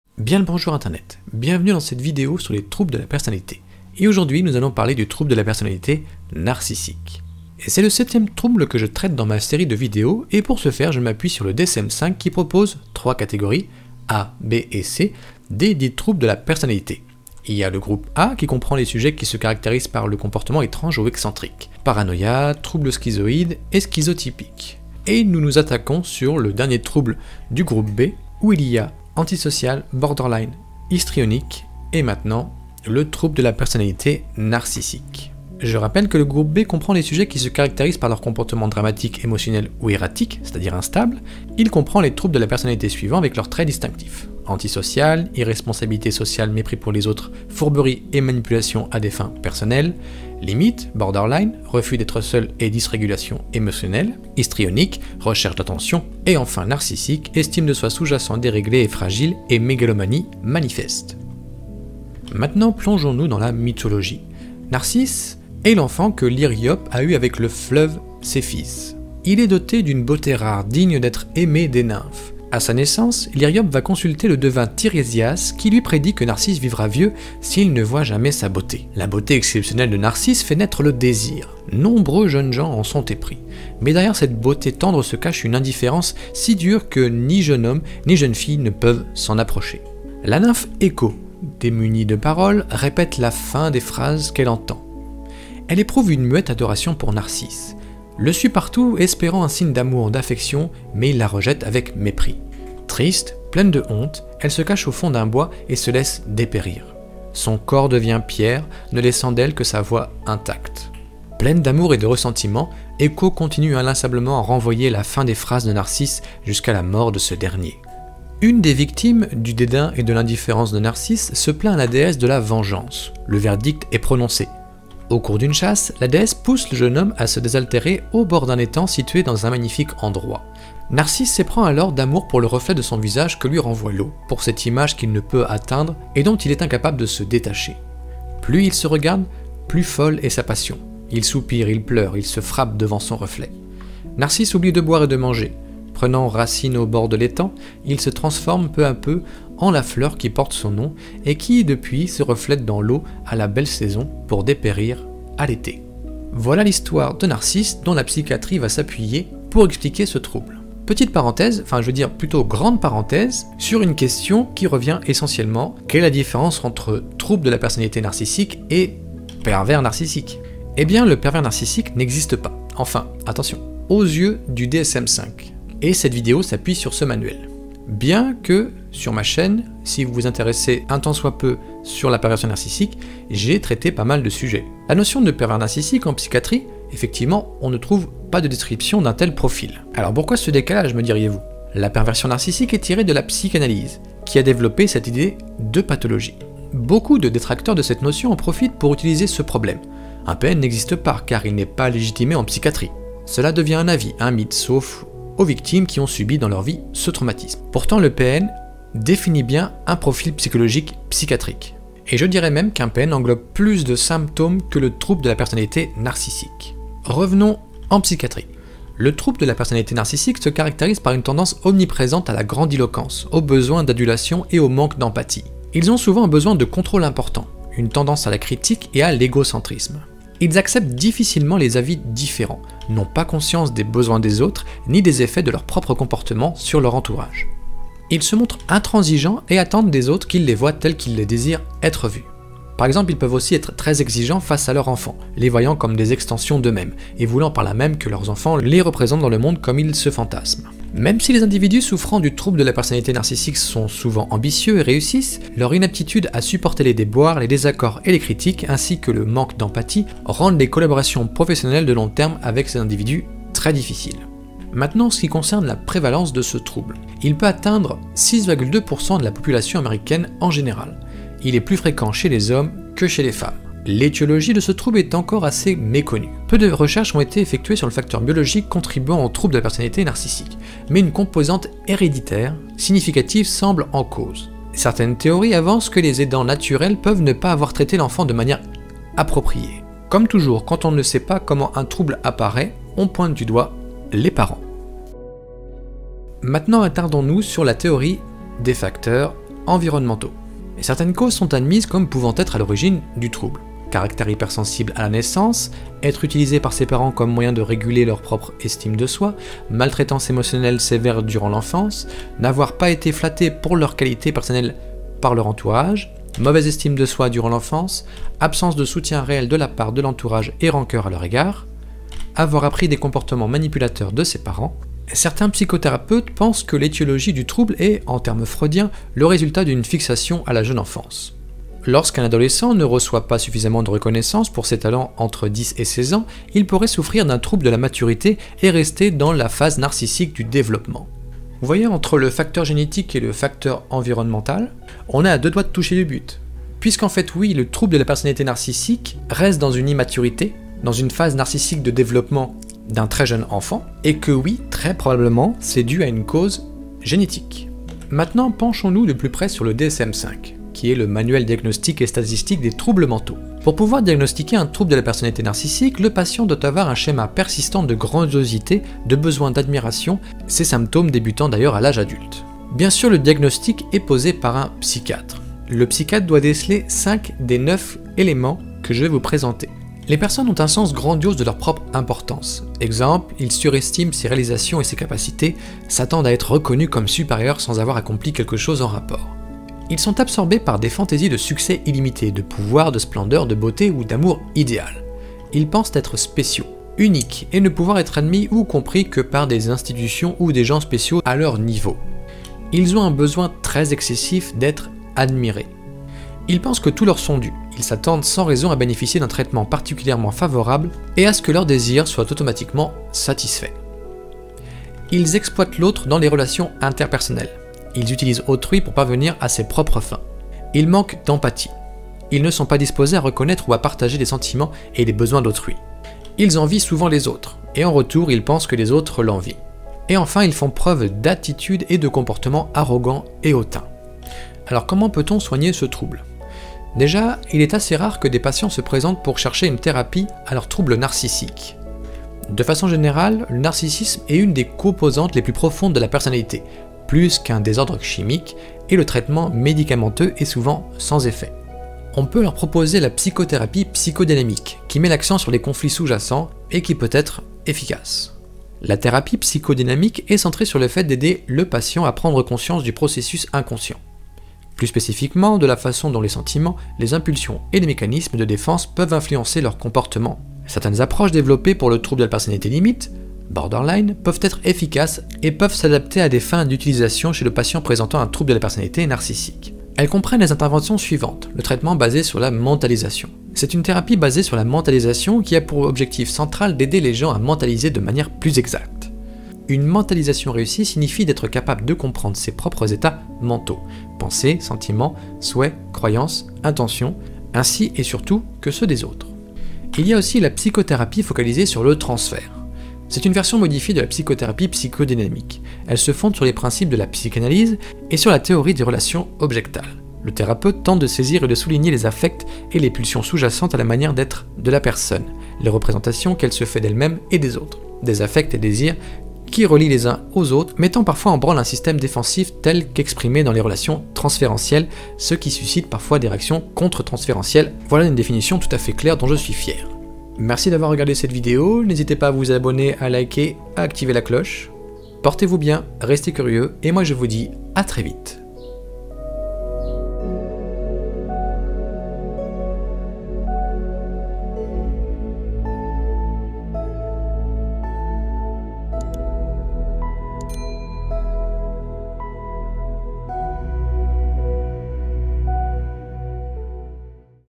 🧠 Trouble de la personnalité Narcissique
Penchons nous maintenant sur le Trouble de la personnalité Narcissique, est ce que c'est la même chose qu'un pervers narcissique ?